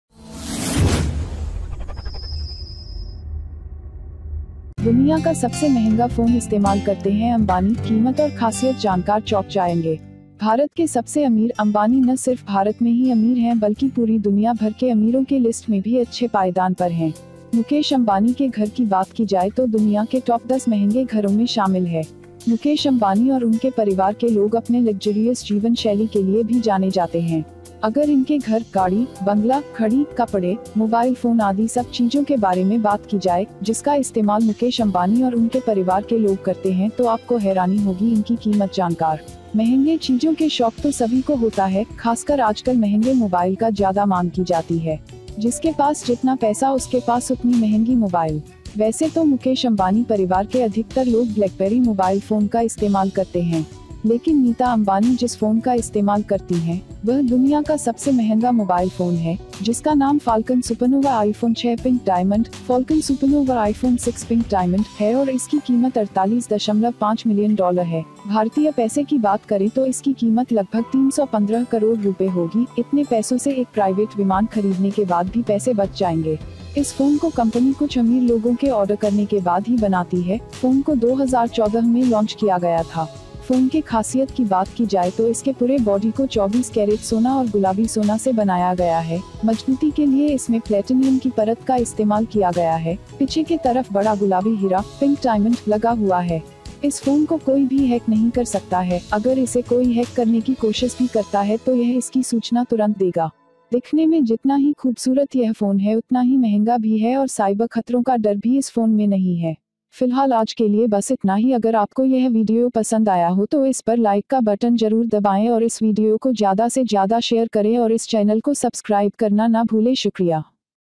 दुनियां का सबसे महंगा फोन इस्तेमाल करते हैं अंबानी, कीमत और खासियत जानकार चौक जायँगे....
India's richest, Ambani, not just the list of the rich are rich, but throughout the whole world in India are also good places. It comes Mukesh Ambani's house, including the world's top 10 most expensive homes.

AK News